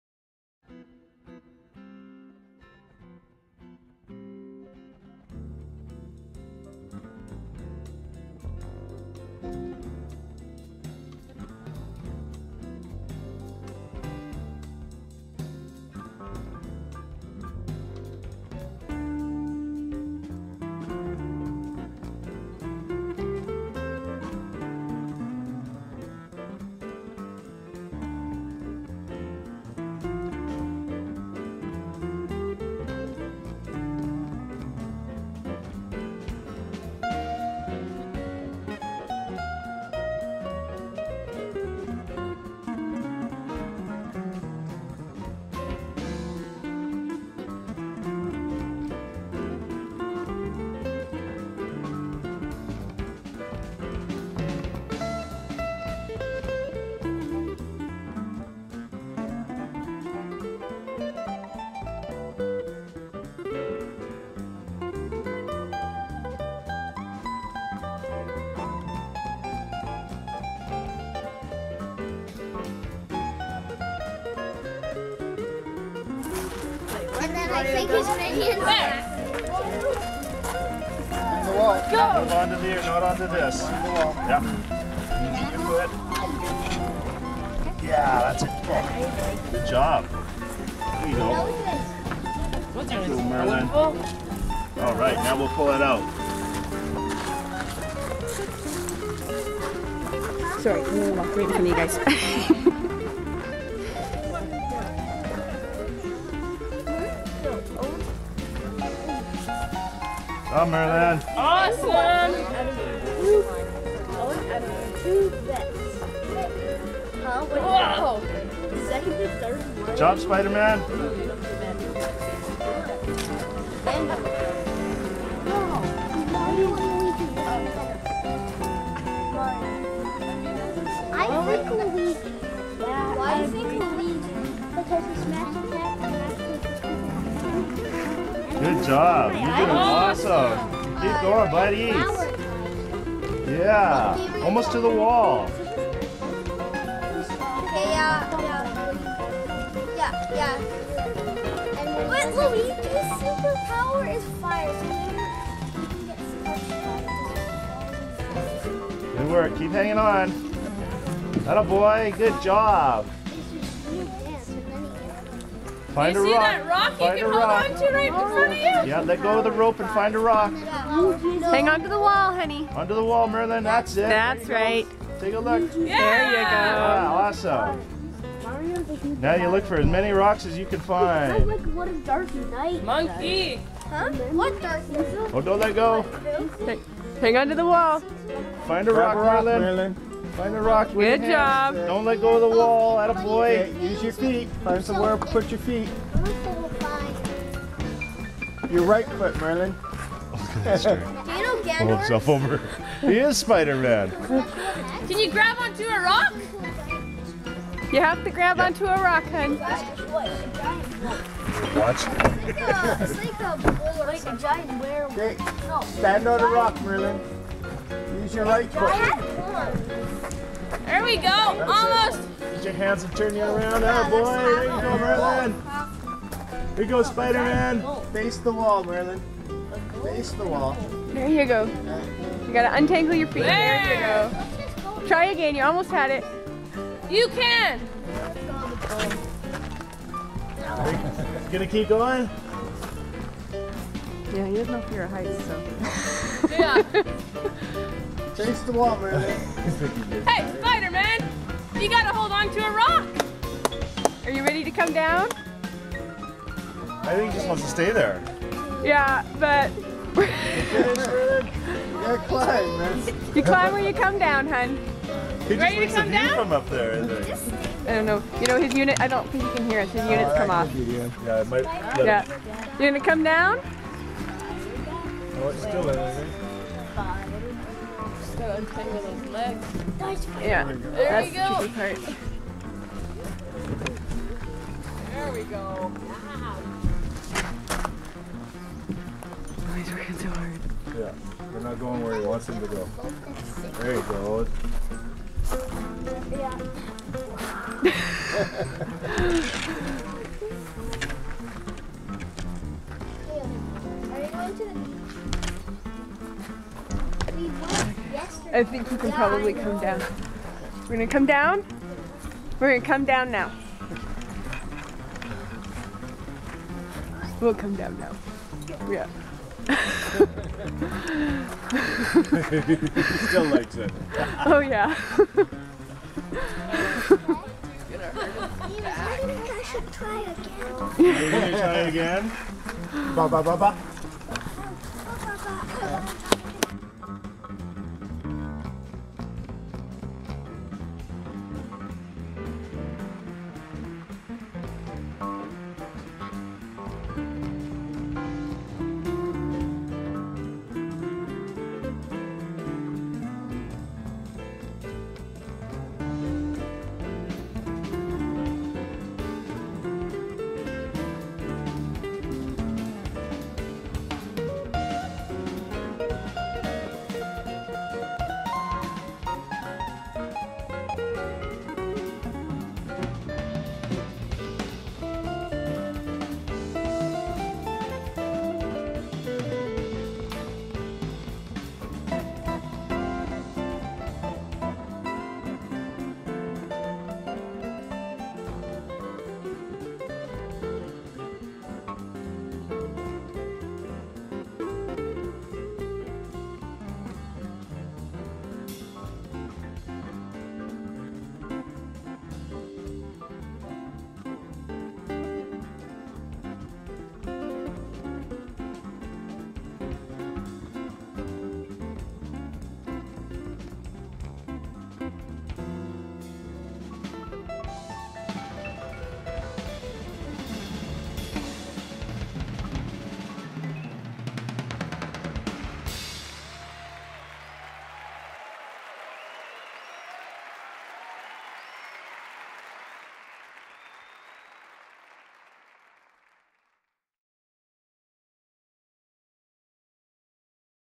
2012 early July
Life on Gabriola and Hornby Island Family Deaf Camp featuring Merlin's first time wall climbing.